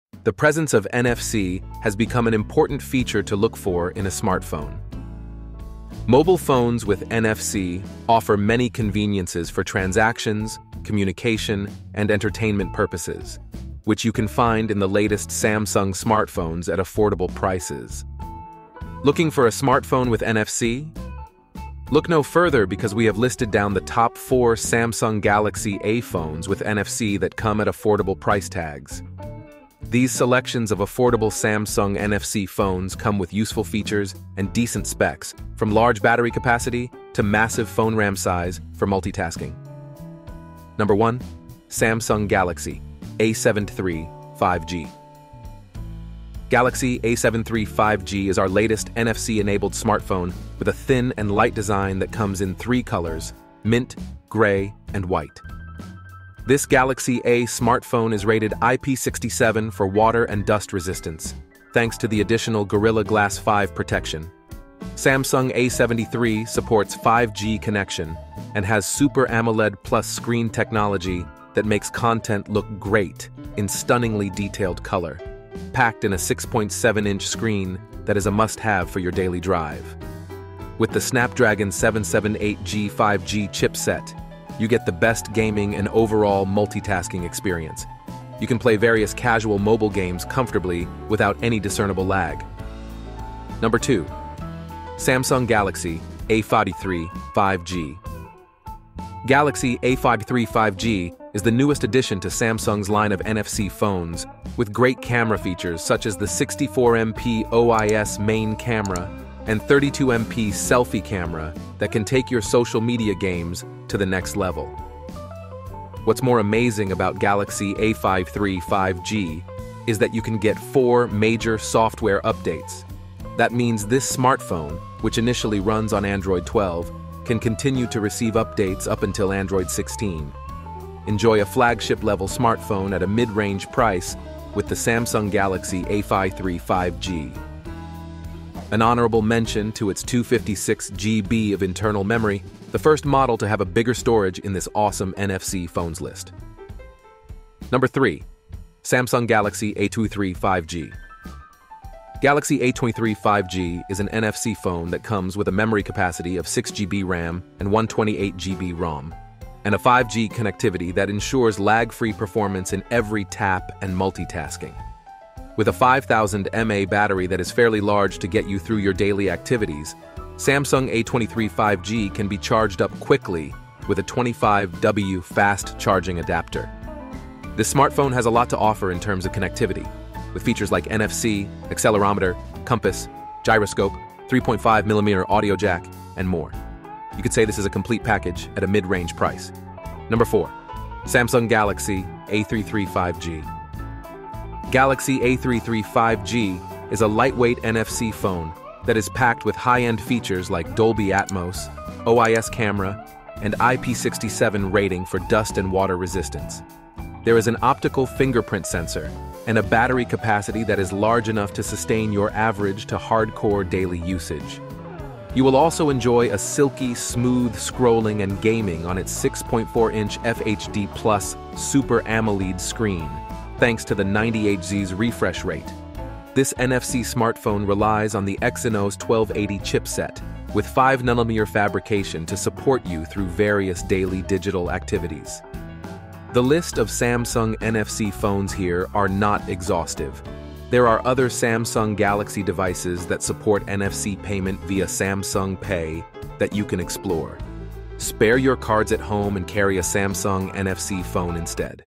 Top 4 Affordable Old Samsung NFC Smartphone to Buy in 2024
Top 4 Affordable old Samsung NFC Smarthones to Buy in 2024
The presence of NFC has become an important feature to look for in a smartphone. Mobile phones with NFC offer many conveniences for transactions, communication, and entertainment purposes, which you can find in the latest Samsung smartphones at affordable prices.

Looking for a smartphone with NFC? Look no further because we have listed down the top 4 Samsung Galaxy A phones with NFC that come at affordable price tags.

These selections of affordable Best phones 2024 Samsung NFC phones come with useful features and decent specs, from large battery capacity to massive phone RAM size for multitasking.

1. Samsung Galaxy A73 5G
2. Samsung Galaxy A53 5G
3. Samsung Galaxy A23 5G
4. Samsung Galaxy A33 5G